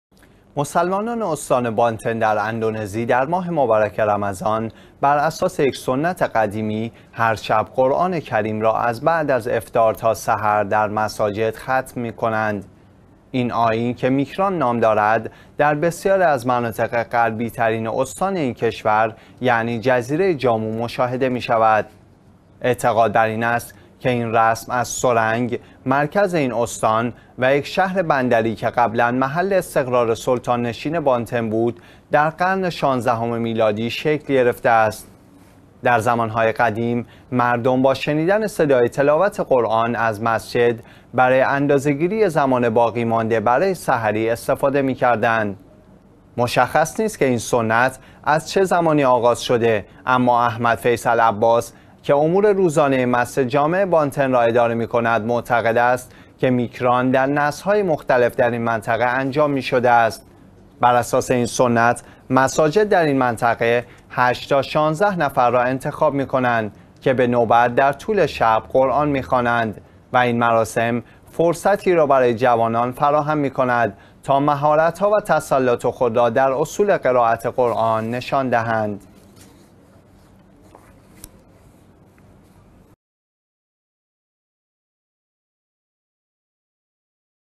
استقبال از سنت ختم قرآن از افطار تا سحر در اندونزی
مسلمانان استان بانتن دراندونزی در ماه مبارک رمضان، بر اساس یک سنت قدیمی، هر شب قرآن کریم را از بعد از افطار تا سحر در مساجد ختم می کنند.
این آیین که میکران نام دارد، در بسیاری از مناطق غربی‌ترین استان این کشور یعنی جزیره جاوا مشاهده می‌شود.
اعتقاد بر این است که این رسم از سرنگ، مرکز این استان و یک شهر بندری که قبلاً محل استقرار سلطان‌نشین بانتن بود، در قرن شانزدهم میلادی شکل گرفته است.
در زمان‌های قدیم، مردم با شنیدن صدای تلاوت قرآن از مسجد برای اندازه‌ گیری زمان باقی مانده برای سحری استفاده می‌کردند.
مشخص نیست که این سنت از چه زمانی آغاز شده، اما "احمد فیصل عباس" که امور روزانه مسجد جامع بانتن را اداره می‌کند، معتقد است که میکران در نسل‌های مختلف در این منطقه انجام می‌شده است.
بر اساس این سنت، مساجد در این منطقه 8 تا ۱۶ نفر را انتخاب می‌كنند كه به نوبت در طول شب قرآن می‌خوانند و این مراسم فرصتی را برای جوانان فراهم می‌كند تا مهارت‌ها و تسلط خود را در اصول قرائت قرآن نشان دهند.